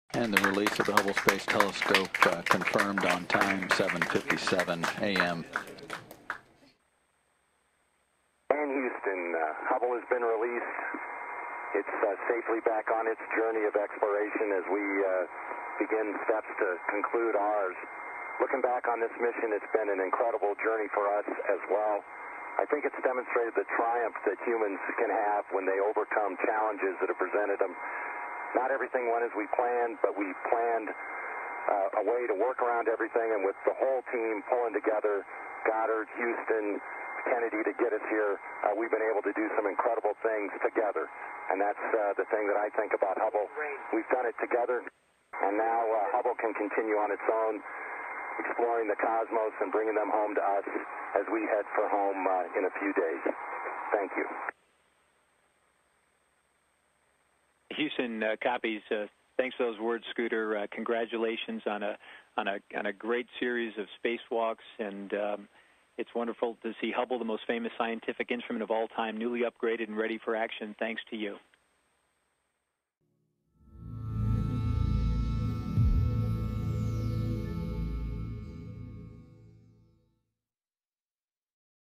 NASA Hubble Team Reactions at the Telescope's Release
The Hubble Space Telescope has been with us for nearly two decades. In that time, its breathtaking images have captured peoples imaginations and its groundbreaking science has revealed some of the many secrets of our universe.

After five spacewalks by the STS-125 mission to repair Hubble, commander Scott "Scooter" Altman confirmed a successful release of the Hubble telescope from the Space Shuttle Atlantis.

As astronaut John Grunsfeld said at the end of the last Hubble spacewalk, Hubble isnt just a satellite. Its about humanitys quest for knowledge. Thanks to this fifth and final shuttle servicing mission, Hubble can continue that quest for years to come.